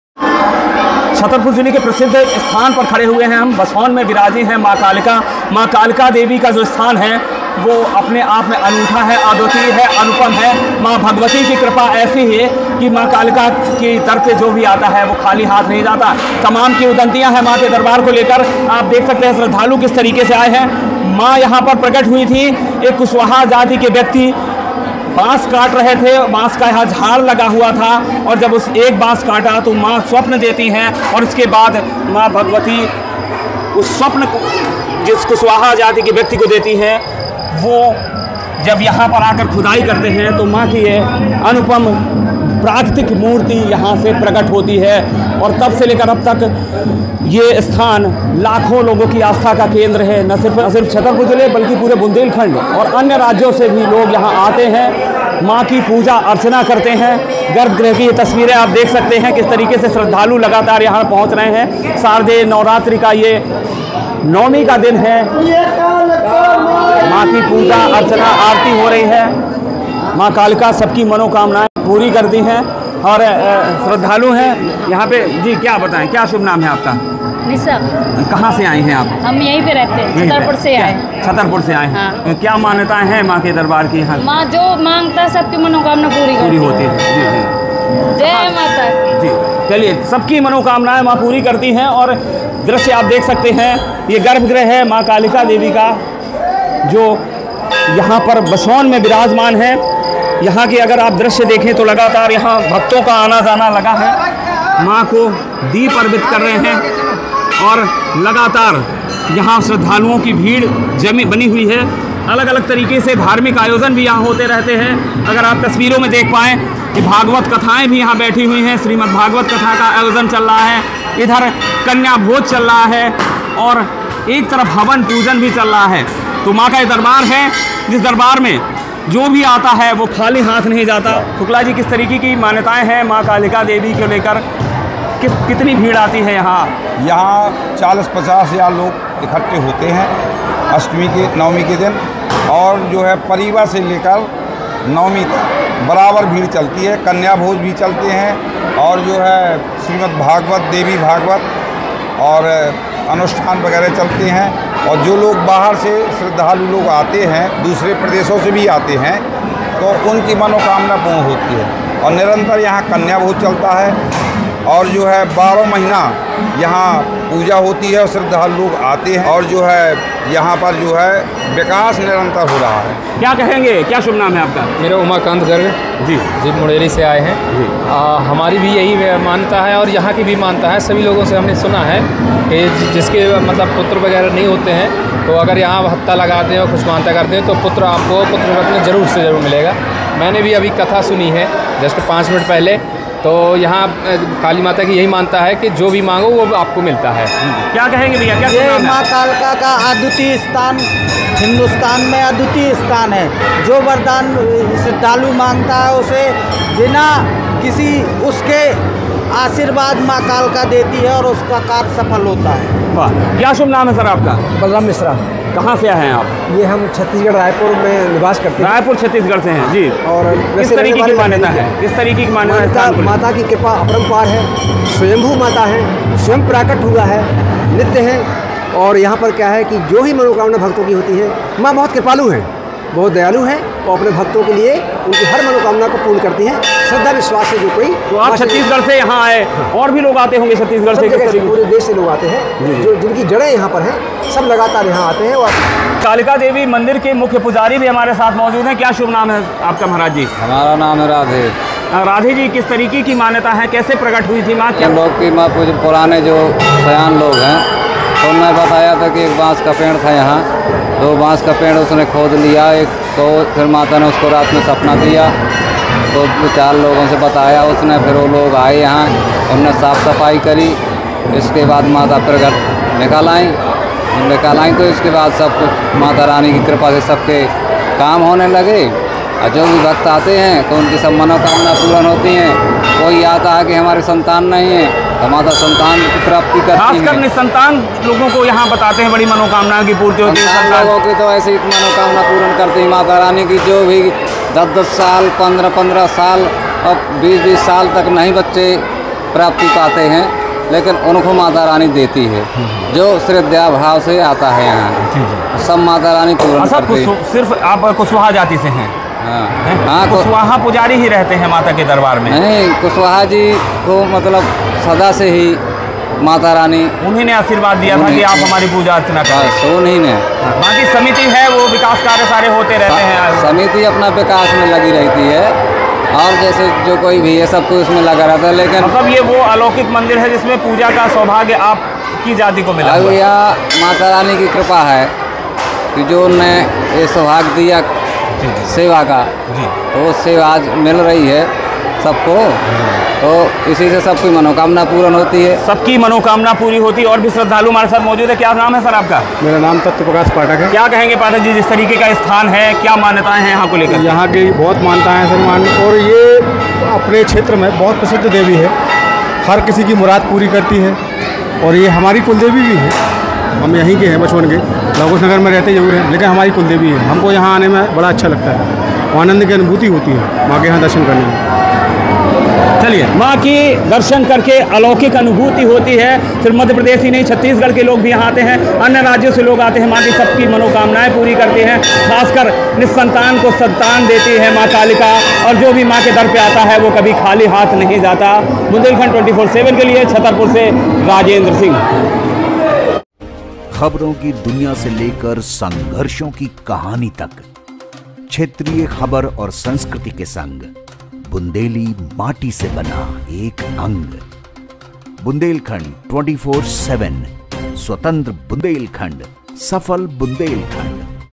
स्वप्न देकर प्रगट हुई थी माँ कालका, निःसन्तान की भरती हैं गोद; यहाँ कुशवाहा जाति के लोग हैं पुजारी
Chhatarpur: स्वप्न देकर प्रगट हुई थी माँ कालका,निःसन्तान की भरतीं हैं गोद,यहाँ कुशवाहा जाती के लोग हैं पुजारी | Bundelkhand 24x7